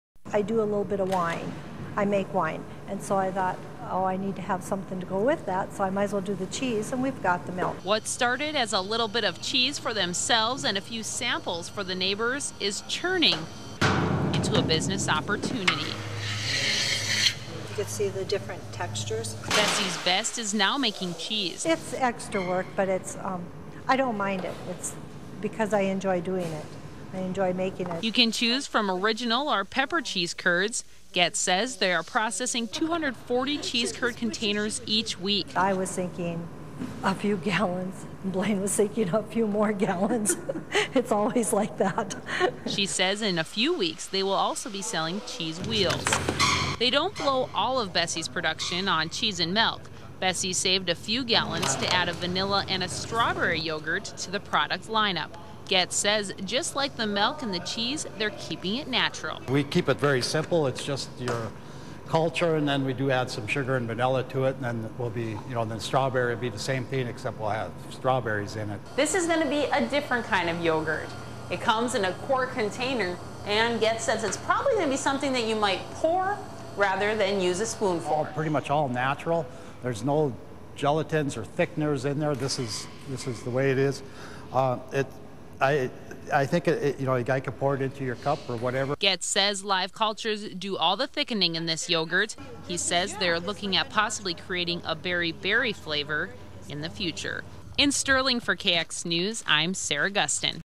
Bessy's Cheese and Yogurt
After about a year after starting their farm-fresh milk bottling operation, Bessy's Best is now making cheese and yogurt.

Shot by Chris.